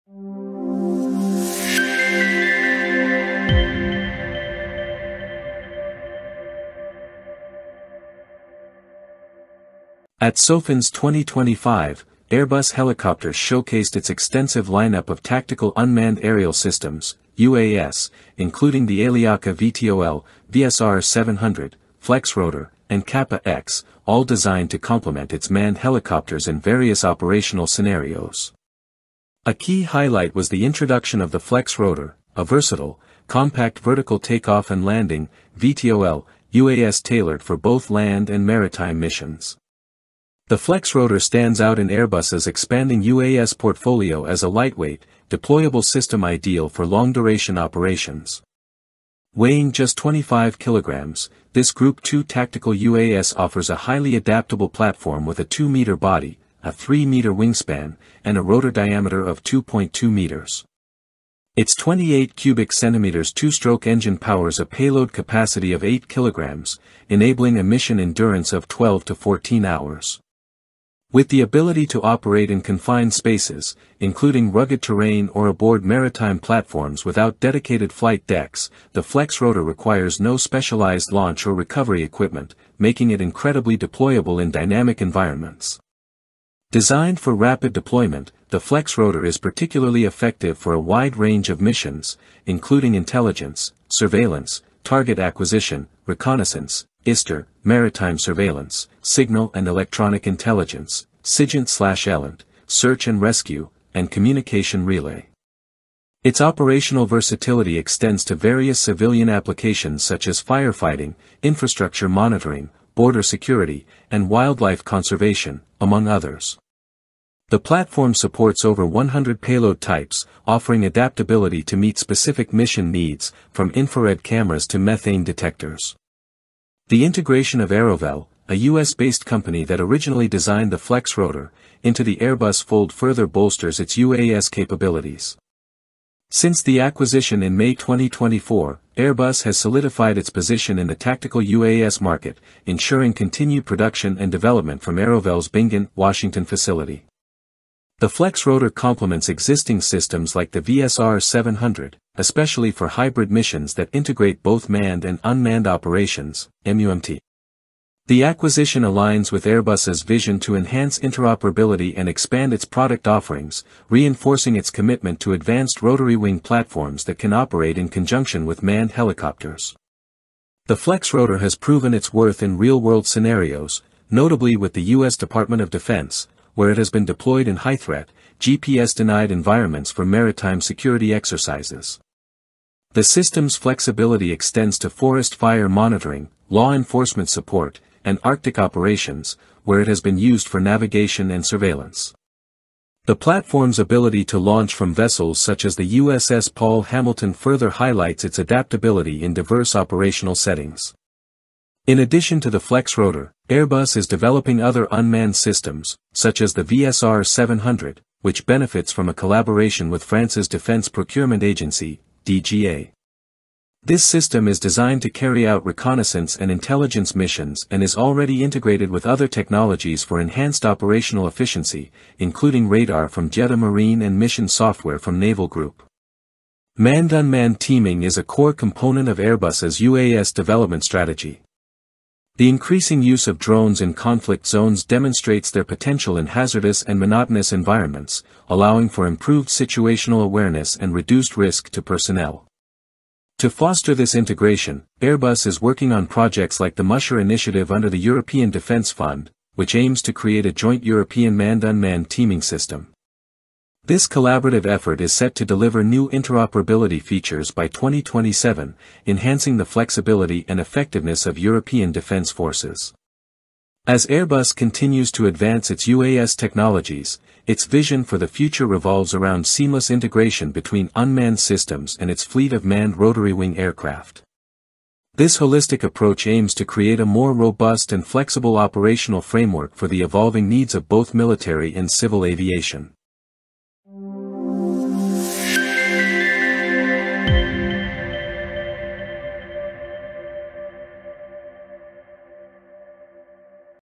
Airbus Expands Tactical UAS Capabilities with Flexrotor at SOFINS 2025
At SOFINS 2025, Airbus Helicopters unveiled its tactical UAS lineup, featuring the Flexrotor, a versatile VTOL system for land and sea operations. The system complements Airbus’ manned helicopters, offering advanced capabilities for a range of military and civilian missions.